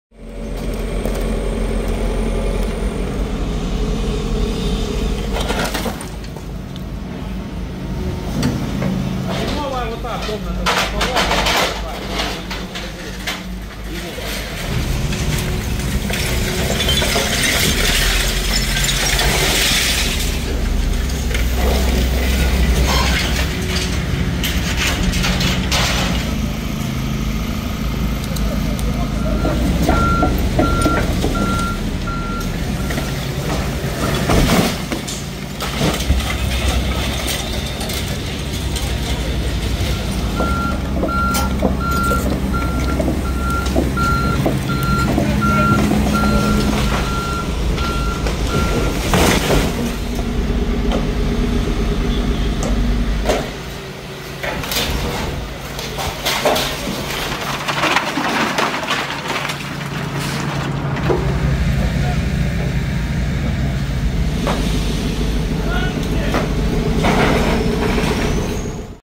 Харків завершені аварійно рятувальні роботи та розбір завалів
Рятувальники Харківського гарнізону ДСНС та 2 Спеціального центру швидкого реагування ДСНС України разом із комунальними службами ліквідували наслідки ворожого влучання по житловому будинку.

Вивезено 240 куб.м. будівельного сміття.

На місці події працювали психологи ДСНС. Вони допомогли 45 особам.